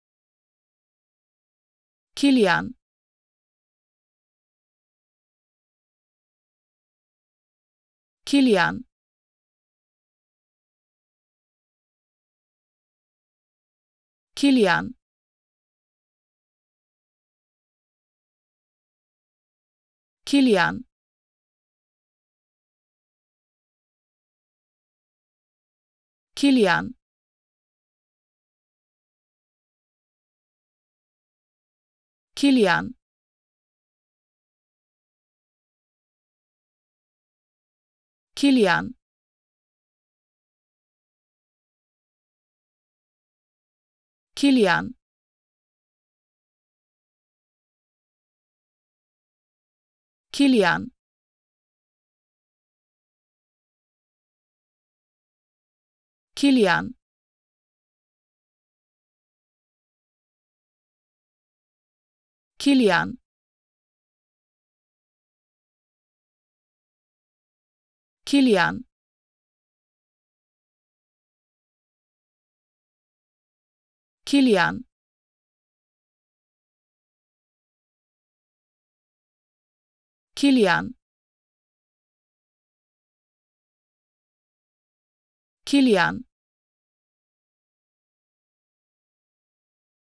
How to Pronounce ''Killian'' Correctly in German
Learn how to say ''Killian'' in German with this short pronunciation tutorial.